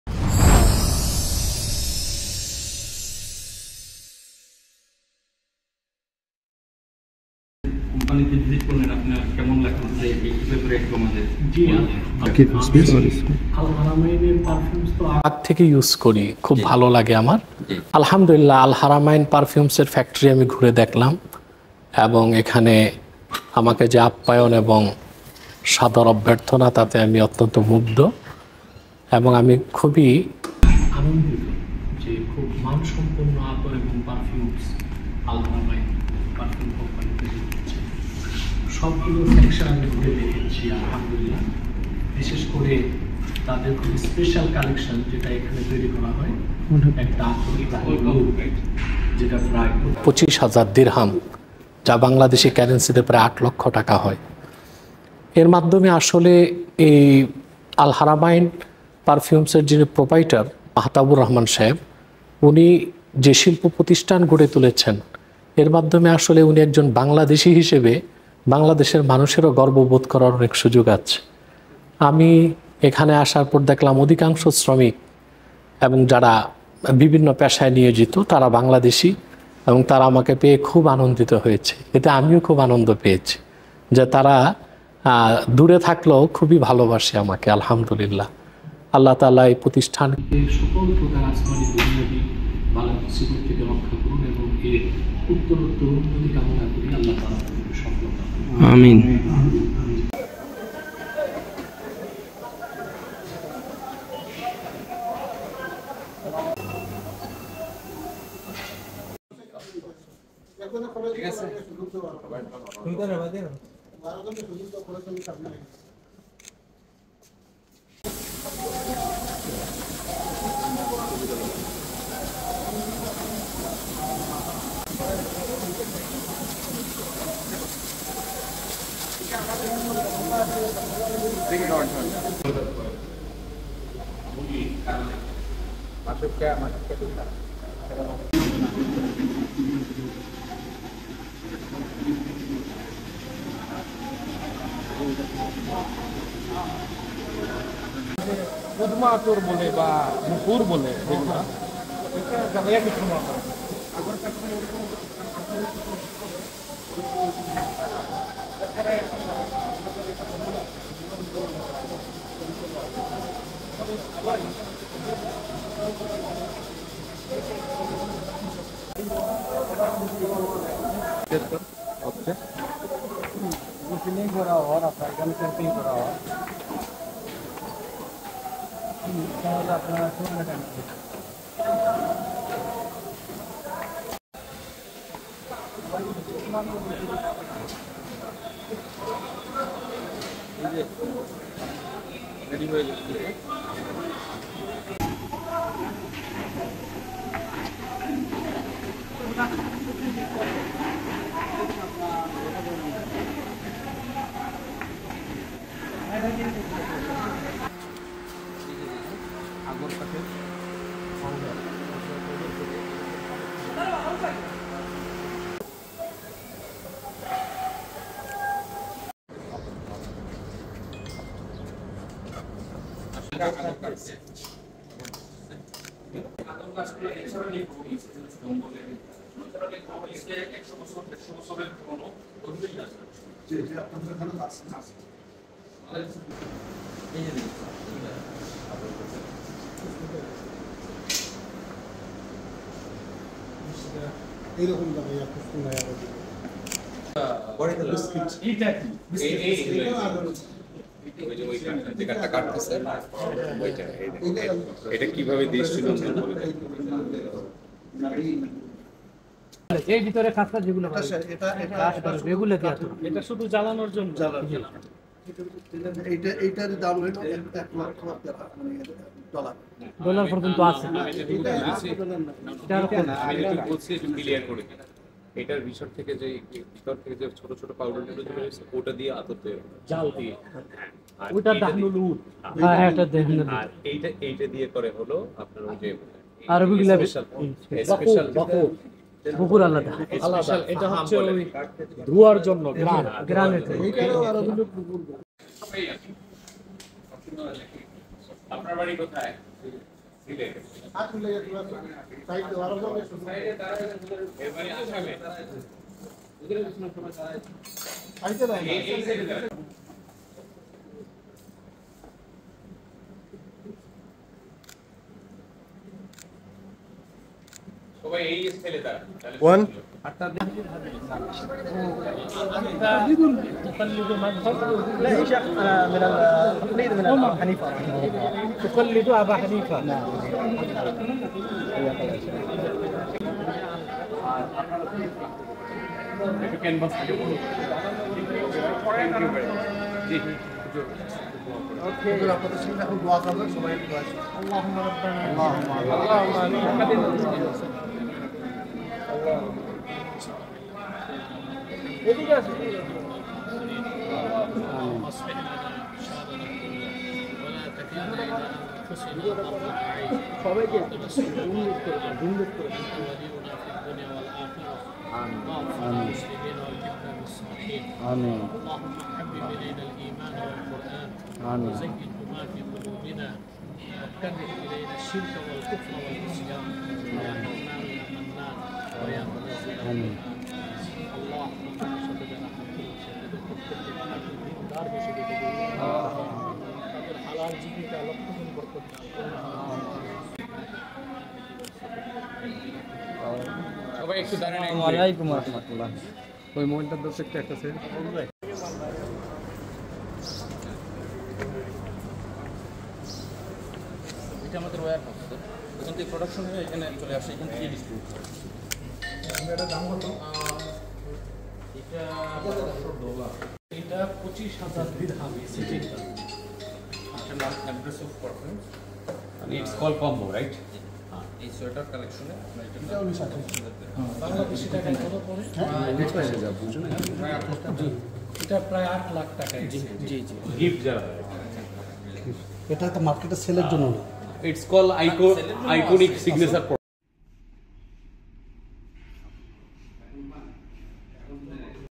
Sheikh Dr. Anayetullah Abbasi visited on the invitation of Al-Haramain Perfumes in the UAE
Sheikh Dr. Anayetullah Abbasi visited and made dua on the invitation of Al-Haramain Perfumes in the United Arab Emirates.
আরব আমিরাতে আল-হারামাইন পারফিউমস কোম্পানির ইনভাইটে আল্লামা আব্বাসী হুজুর পরিদর্শন ও দোয়া করেন।